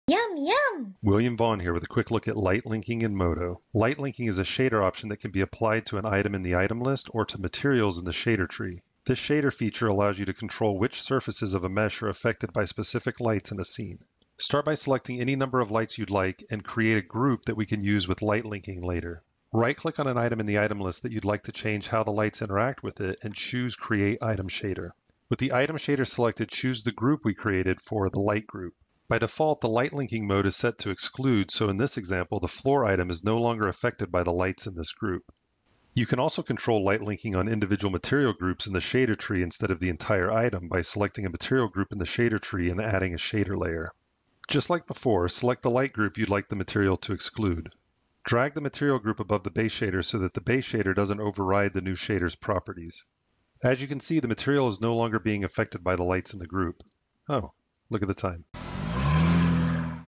MODO | Introduction to Light Linking
Light Linking is a shader option that allows you to control which surfaces of a mesh are affected by specific lights in a scene. This quick video introduces you to this incredibly powerful feature and shows multiple ways of using it.